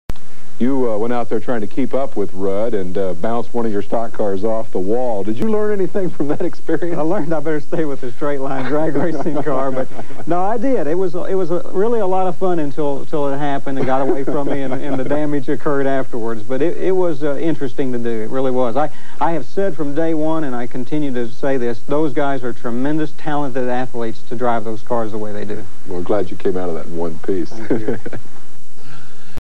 Kenny Bernstein's NASCAR Test and Crash, 1988
In early 1988 at the Atlanta Motor Speedway, NHRA champion Kenny Bernstein tested one of the NASCAR Winston Cup cars that he owned. Following his driver Ricky Rudd through turn 2, Bernstein lost control and wrecked hard.